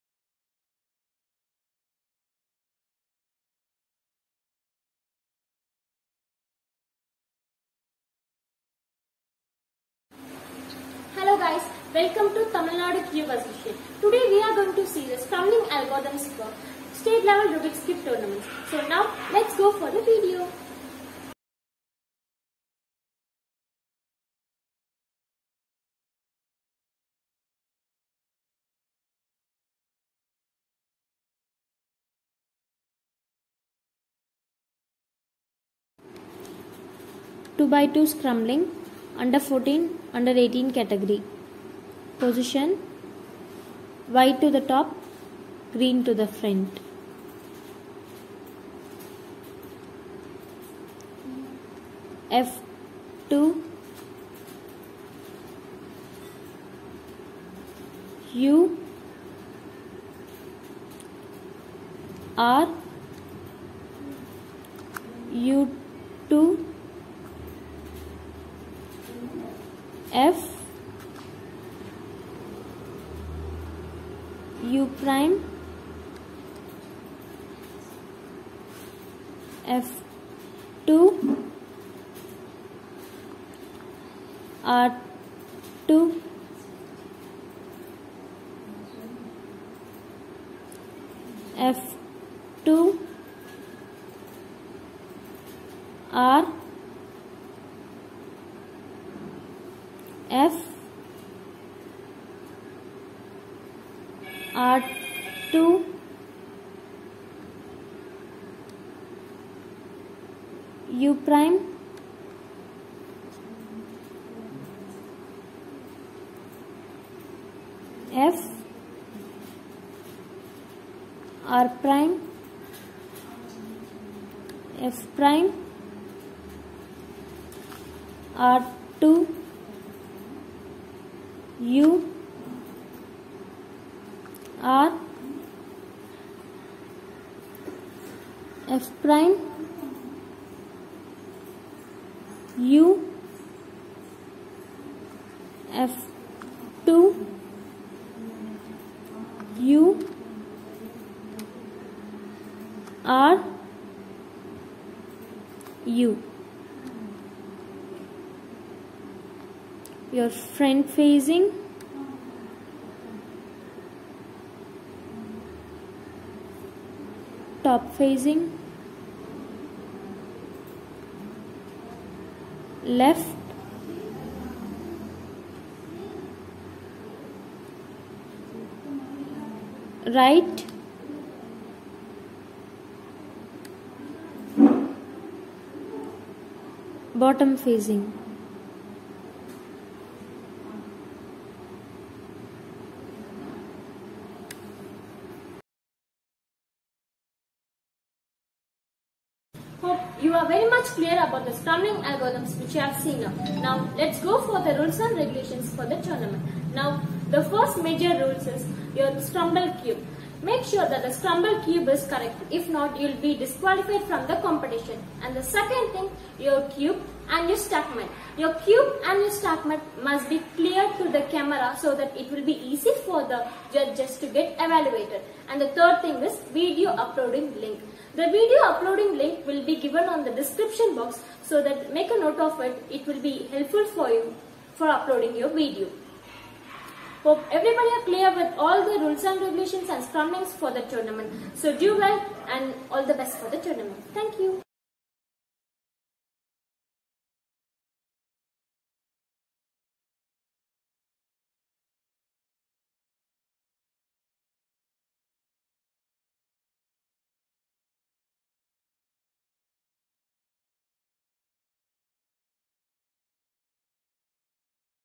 2x2 cube Scrambling Algorithm for State Level Cube Tournament 2021 UNDER - 14, UNDER - 18
2x2 cube Scrambling Algorithm for State Level Cube Tournament 2021

UNDER - 14, UNDER - 18

Video Uploading link will be active on 27th June 2021 from 9 a.m.